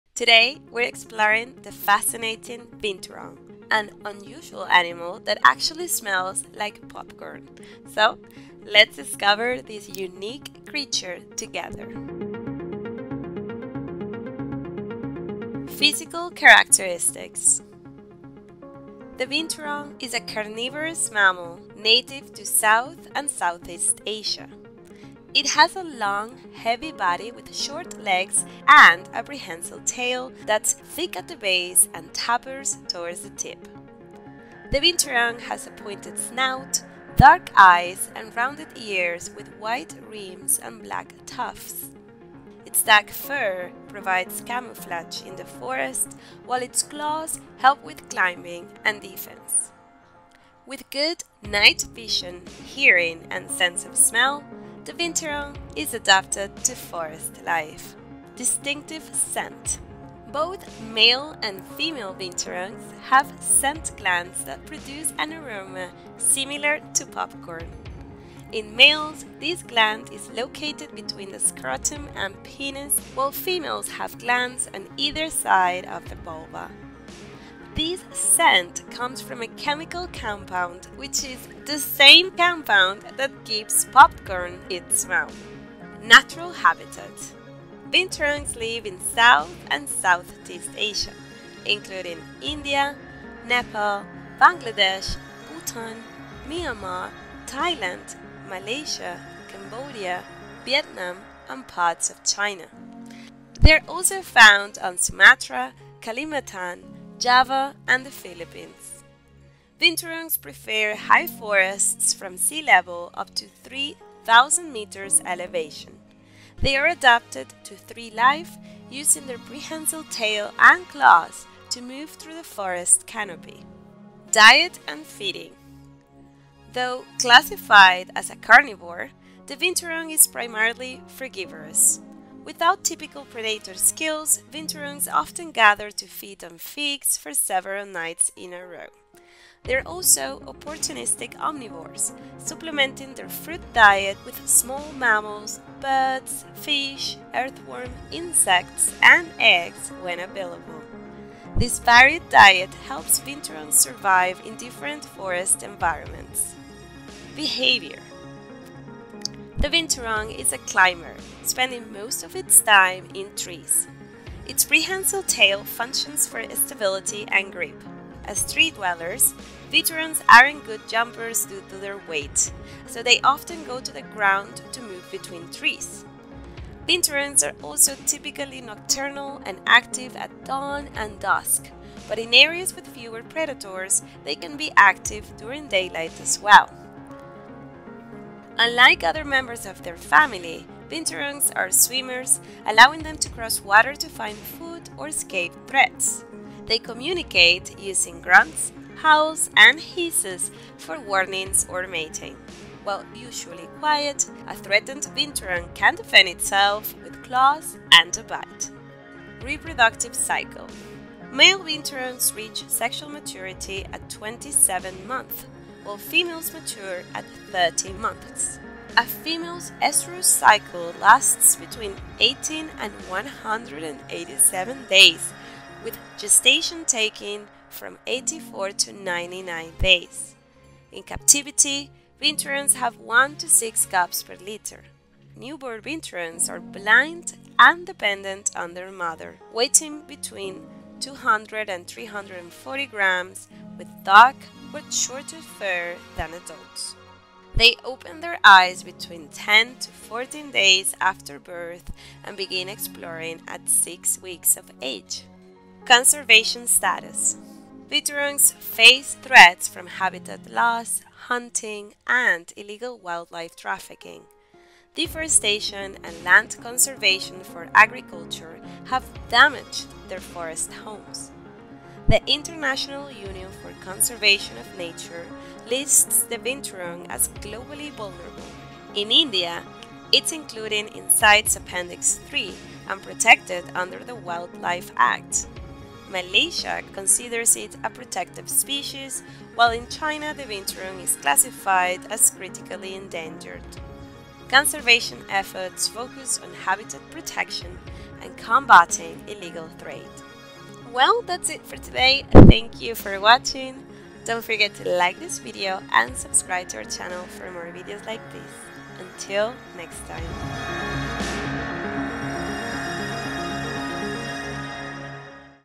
What is a BINTURONG? 🦝 The Bearcat That Smells Like Popcorn!
🦝 Did you know that the BINTURONG is a mammal that smells like popcorn? 🐾 In this video by thedailyECO, we reveal fascinating facts about this curious Southeast Asian animal. Learn where the binturong lives, what it eats, how it behaves, and why it’s endangered. Discover its most surprising characteristics and why this unique species needs protection. Don’t miss out on learning about this amazing animal.

00:00 Introduction
00:15 Physical Characteristics
01:04 Distinctive Scent
01:24 Natural Habitat
01:55 Diet and Feeding
02:25 Behavior Patterns
03:14 Reproductive Cycle
03:49 Conservation Status